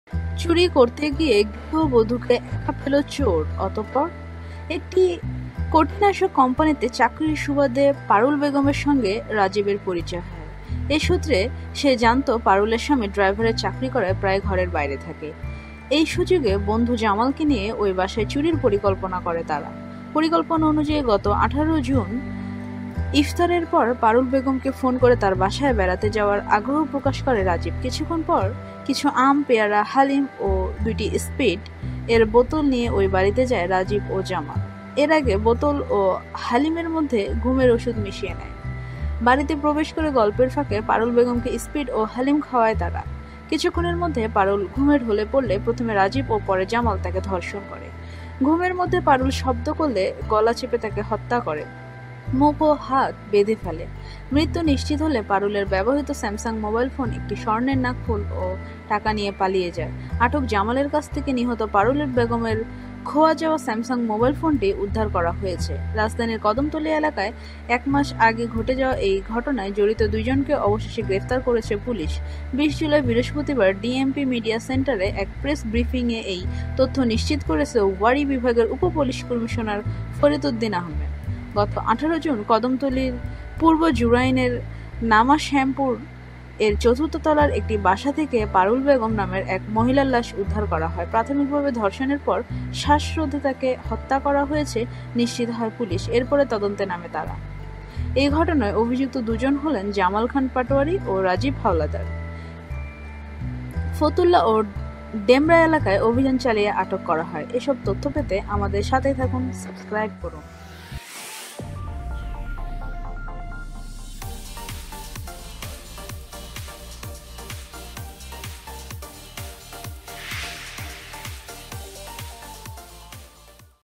বাসায় বৌ কে একা রেখে বাহিরে যাচ্ছেন, তাহলে এই ভিডিওটি দেখুন
একটি কটিনাশক কোম্পানিতে চাকুরির সুবাদে পারুল বেগমের সঙ্গে রাজীবের পরিচয় হয়। এসূত্রে সে জানতো পারুলের স্বামী ড্রাইভারের চাকুরি করায় প্রায়ই ঘরের বাইরে থাকে। এই সুযোগে বন্ধু জামালকে নিয়ে ওই বাসায় চুরির পরিকল্পনা করে তারা।

পরিকল্পনা অনুযায়ী গত ১৮ জুন ইফতারের পর পারুল বেগমকে ফোন করে তার বাসায় বেড়াতে যাওয়ার আগ্রহ প্রকাশ করে রাজীব। কিছুক্ষণ পর কিছু আম, পেয়ারা, হালিম ও দুইটি স্পীড (পানীয়) এর বোতল নিয়ে ওই বাড়িতে যায় রাজীব ও জামাল। এর আগে বোতল ও হালিমের মধ্যে ঘুমের ঔষধ মিশিয়ে নেয়।

বাড়িতে প্রবেশ করে গল্পের ফাঁকে পারুল বেগমকে স্পীড ও হালিম খাওয়ায় তারা। কিছুক্ষণের মধ্যে পারুল ঘুমে ঢলে পড়লে প্রথমে রাজীব ও পরে জামাল করে। ঘুমের মধ্যে পারুল শব্দ করলে মুখ ও হাত বেঁধে ফেলে। নিশ্চিত হলে পারুলের ব্যবহৃত স্যামসং মোবাইল ফোন, একটি স্বর্ণের নাকফুল ও টাকা নিয়ে পালিয়ে যায়।